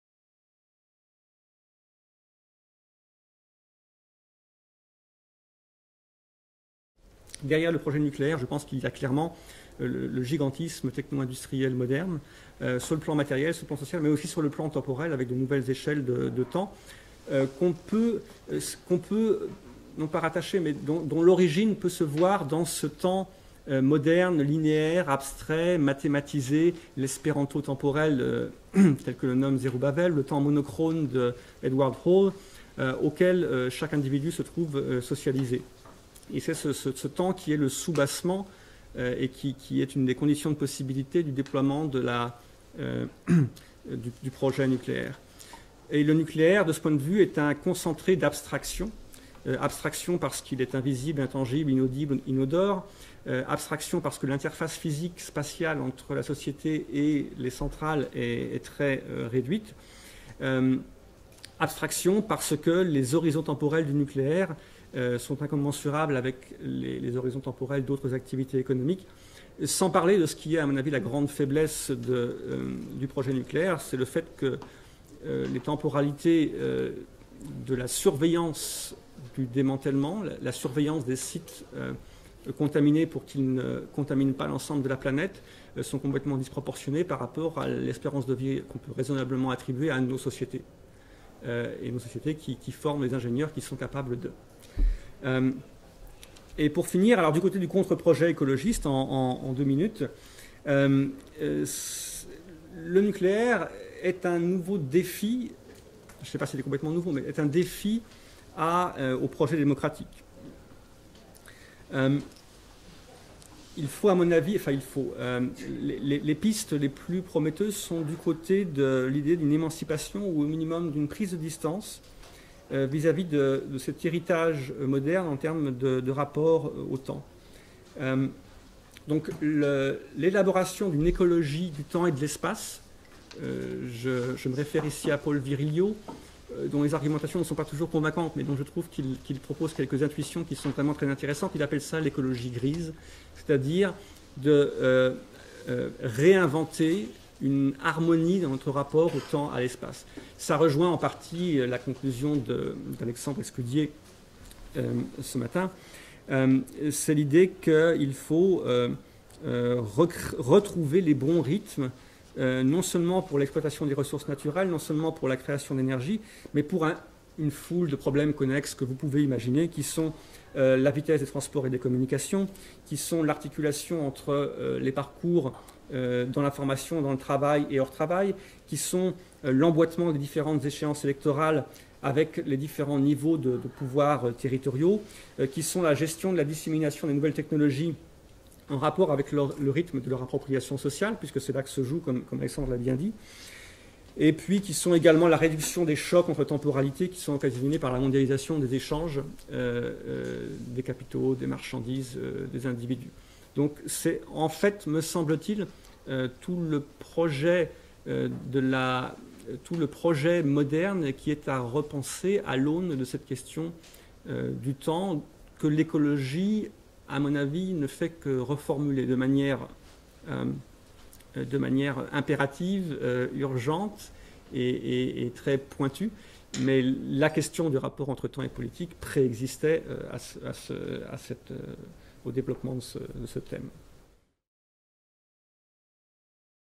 Kychtym, Tchernobyl, Fukushima : demain meurt-il jamais ?
Le projet nucléaire s'inscrit dans un temps très long. La dégradation des déchets nucléaire se compte en milliers, voire en millions d'années, alors que nos sociétés évoluent dans des temporalités beaucoup plus courtes. Comment dès lors gérer ce projet, sachant qu'il dépassera largement la survie de nos sociétés qui produisent les technologies, les savoirs et les individus capables de gérer le traitement des substances radioactives?